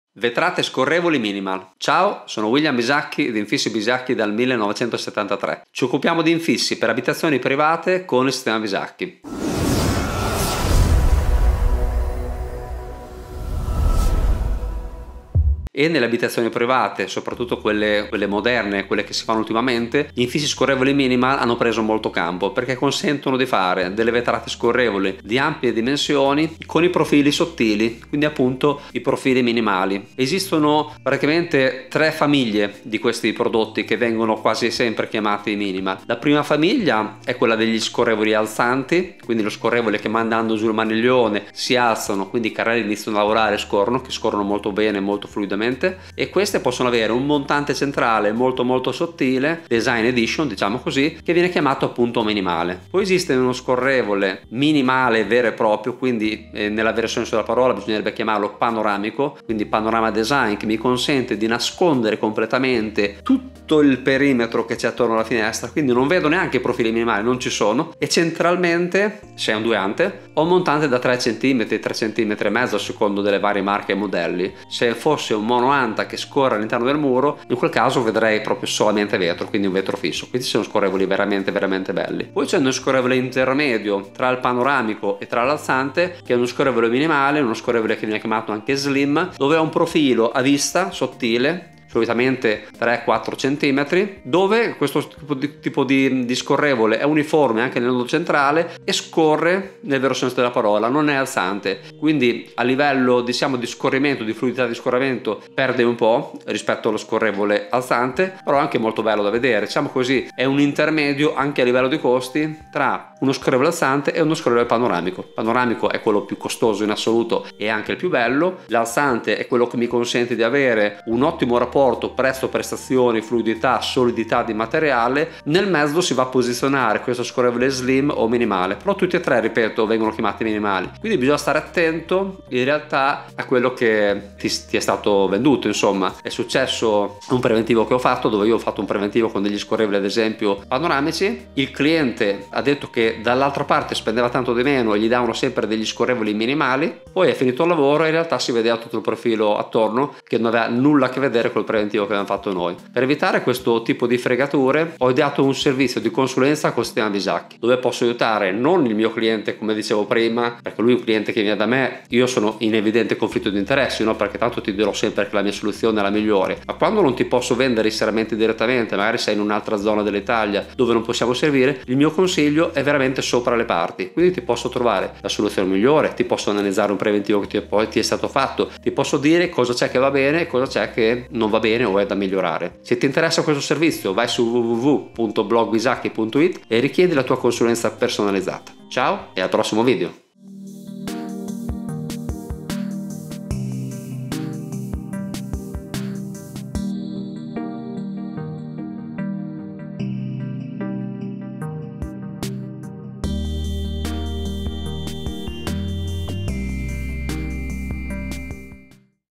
Vetrate scorrevoli minimal .
0:00 infissi scorrevoli minimal
0:30 tre tipologie di scorrevoli minimali
1:52 prezzi scorrevoli minimal
2:33 consulenza su scorrevoli minimal

Ciao sono William Bisacchi di Infissi Bisacchi dal 1973, ci occupiamo di infissi per abitazioni private con il Sistema Bisacchi, e nelle abitazioni private, soprattutto quelle quelle moderne, quelle che si fanno ultimamente, gli infissi scorrevoli minimal hanno preso molto campo perché consentono di fare delle vetrate scorrevoli di ampie dimensioni con i profili sottili, quindi appunto i profili minimali. Esistono praticamente tre famiglie di questi prodotti che vengono quasi sempre chiamati minimal: la prima famiglia è quella degli scorrevoli alzanti, quindi lo scorrevole che mandando giù il maniglione si alzano, quindi i carrelli iniziano a lavorare e scorrono, che scorrono molto bene e molto fluidamente, e queste possono avere un montante centrale molto molto sottile, design edition diciamo così, che viene chiamato appunto minimale; poi esiste uno scorrevole minimale vero e proprio, quindi nella versione sulla parola bisognerebbe chiamarlo panoramico, quindi panorama design, che mi consente di nascondere completamente tutto il perimetro che c'è attorno alla finestra, quindi non vedo neanche profili minimali, non ci sono, e centralmente ci sono due ante o un montante da 3 cm, 3 centimetri e mezzo, a seconda delle varie marche e modelli. Se fosse un mono anta che scorre all'interno del muro in quel caso vedrei proprio solamente vetro, vetro fisso, quindi sono scorrevoli veramente veramente belli, Poi c'è lo scorrevole intermedio, tra il panoramico e tra l'alzante, che è uno scorrevole minimale, uno scorrevole che viene chiamato anche slim, dove ha un profilo a vista sottile, solitamente 3-4 cm, dove questo tipo di scorrevole è uniforme, anche nel nodo centrale, e scorre nel vero senso della parola, non è alzante, quindi a livello di scorrimento, di fluidità di scoramento perde un po' rispetto allo scorrevole alzante, però è anche molto bello da vedere, diciamo così, è un intermedio anche a livello di costi tra uno scorrevole alzante e uno scorrevole panoramico; il panoramico è quello più costoso in assoluto, e anche il più bello, l'alzante è quello che mi consente di avere un ottimo rapporto prezzo-prestazioni-fluidità-solidità di materiale, nel mezzo si va a posizionare questo scorrevole slim, o minimale, però tutti e tre ripeto vengono chiamati minimali. Quindi, bisogna stare attenti in realtà a quello che ti è stato venduto insomma, è successo ad un preventivo che ho fatto, dove avevo fatto un preventivo con degli scorrevoli, ad esempio, panoramici, il cliente ha detto che dall'altra parte spenderà tanto di meno e gli davano sempre degli scorrevoli minimali, poi è finito il lavoro, e in realtà si vedeva il profilo attorno che non ha nulla a che vedere col preventivo che abbiamo fatto noi. Per evitare questo tipo di fregature ho ideato un servizio di consulenza con Sistema Bisacchi, dove posso aiutare non il mio cliente, come dicevo prima, perché lui è un cliente che viene da me, io sono in evidente conflitto d'interessi, perché tra l'altro ti dirò sempre che la mia soluzione è la migliore, ma quando non ti posso vendere i serramenti direttamente, magari sei in un'altra zona dell'Italia dove non ti possiamo seguire, il mio consiglio è veramente sopra le parti, quindi ti posso trovare la soluzione migliore, ti posso analizzare un preventivo che poi ti è stato fatto, ti posso dire cosa c'è che va bene e cosa c'è che non va bene o è da migliorare.